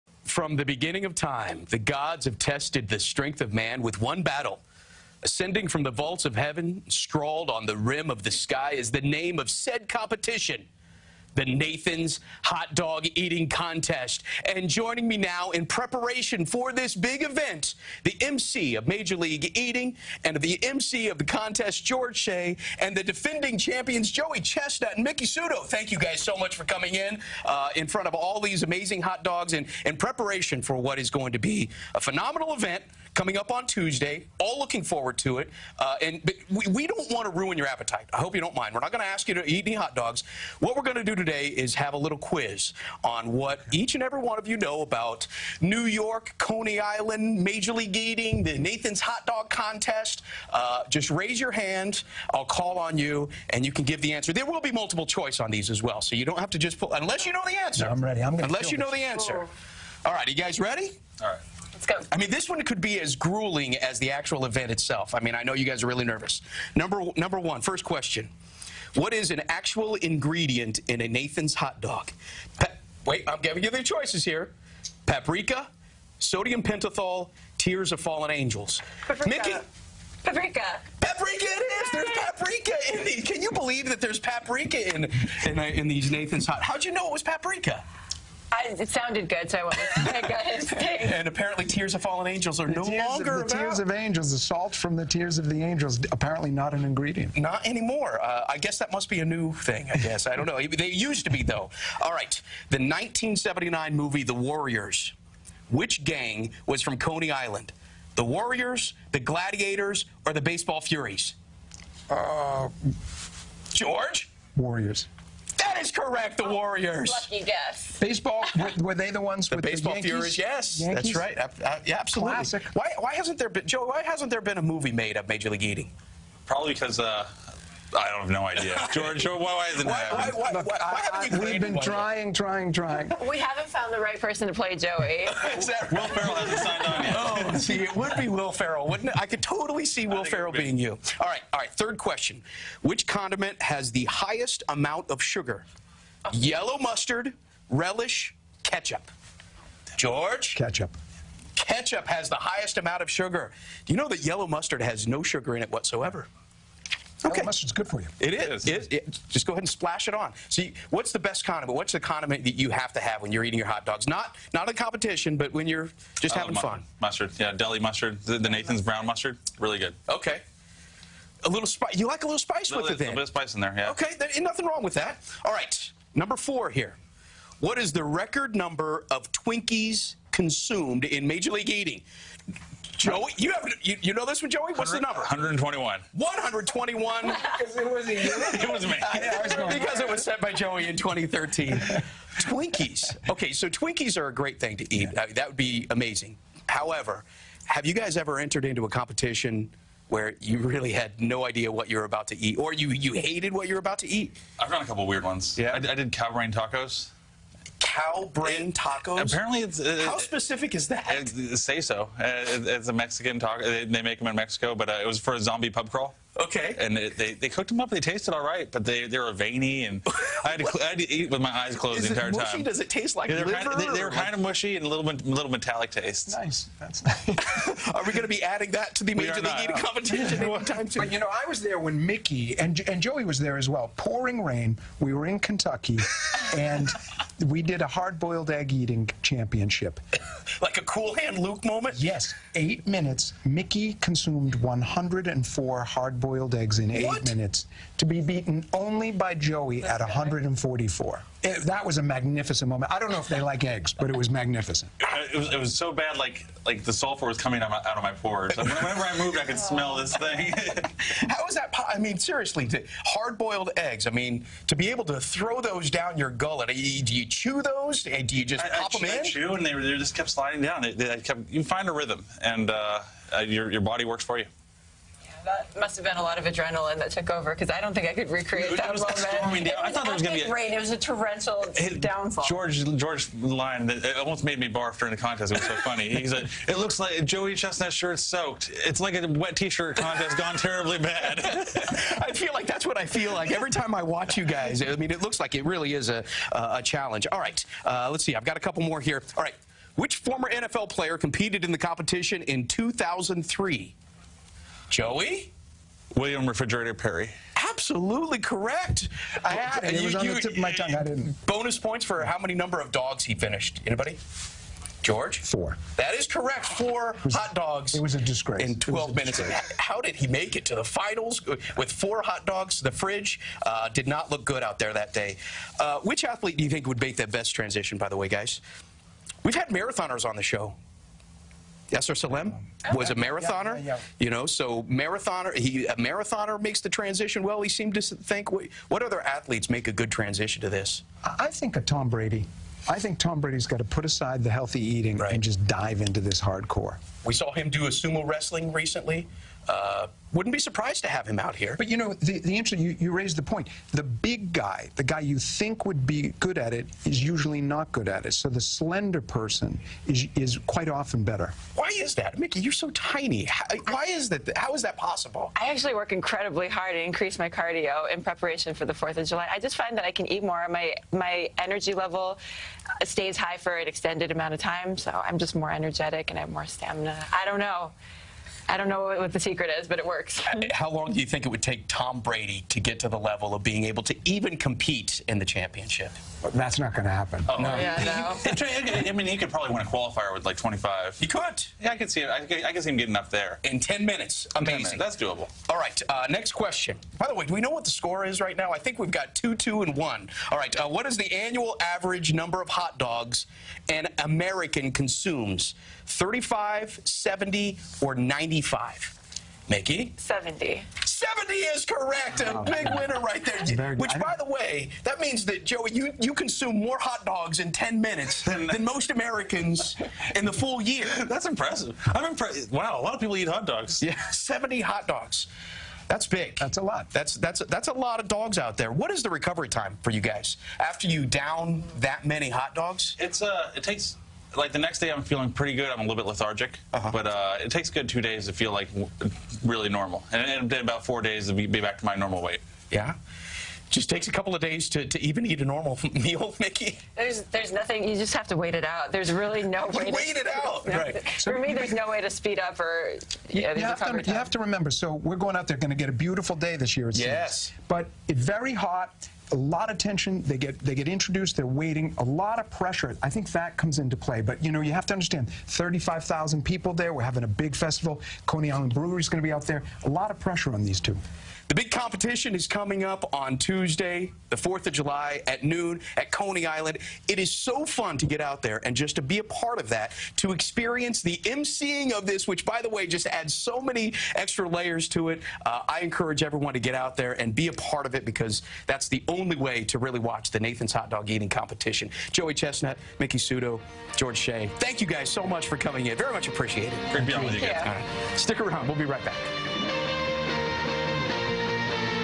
A Quiz With The Hot Dog Eating Champions
Nathan's Hot Dog Eating Contest champions Joey Chestnut and Miki Sudo, and emcee George Shea, join Steve Overmyer for a little quiz.